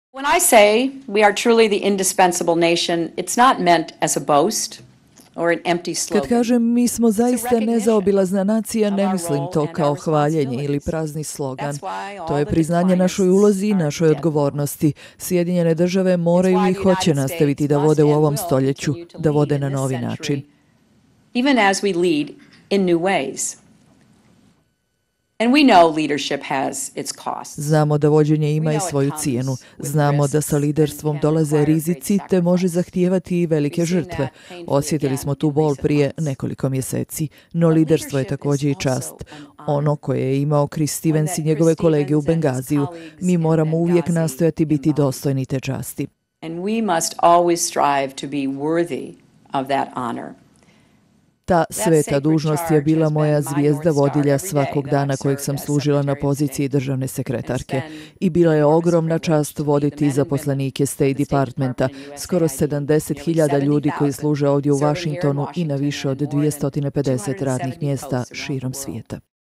Oproštaj Hillary Clinton: Nastavićemo predvoditi svijet
Američka državna tajnica Hillary Clinton izjavila je u svom posljednjem javnom govoru prije napuštanja funkcije, da će SAD nastaviti da predvode i u 21. stoljeće. Video: AP, 1. februar 2013.